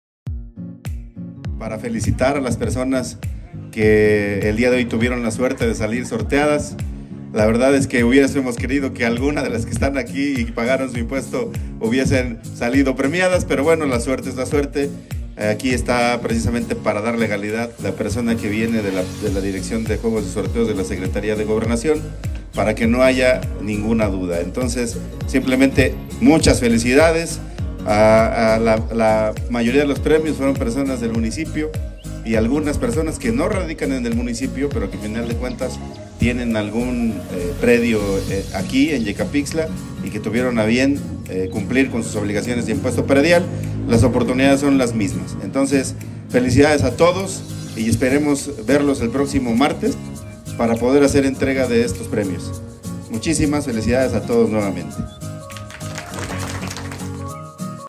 PREMIA YECAPIXTLA CONTRIBUCIÓN DE CIUDADANÍA
Este año, el Ayuntamiento de Yecapixtla, que preside el alcalde Heladio Rafael Sánchez Zavala, realizó el sorteo de motocicletas y aparatos electrónicos para reconocer la aportación que realizan los ciudadanos a través del pago de impuesto predial.